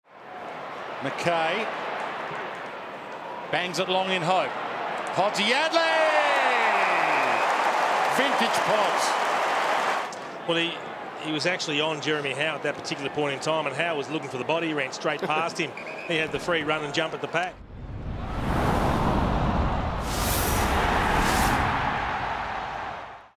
AFL Mark of the Year - James Podsiadly Round 7 2014
Adelaide's James Podsiadly is a round seven Lifebroker Mark of the Year nominee.